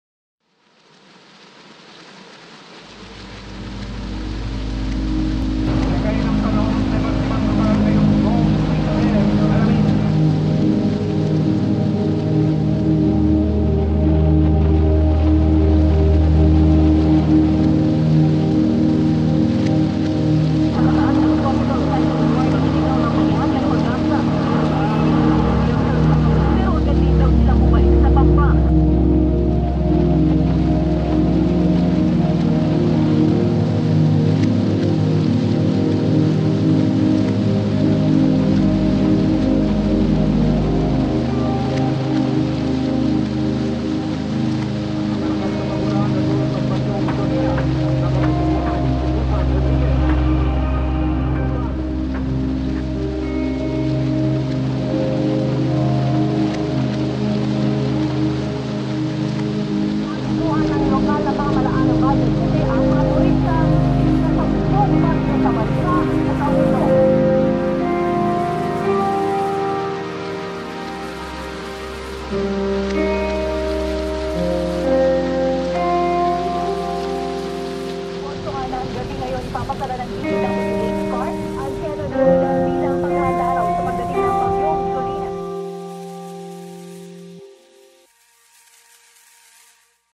'Light Rain'
Hustle and bustle of city streets,
A storm is coming, people in fleets.
Lights hit the glass, red, green and yellow
Back seated, the streets are dark and mellow
Rain drops, they fell in rhythmic proportion
Dancing of lights, fading slowly in motion.

-Hi, this is Kursten Mori, and this is a short film i made while stuck in traffic.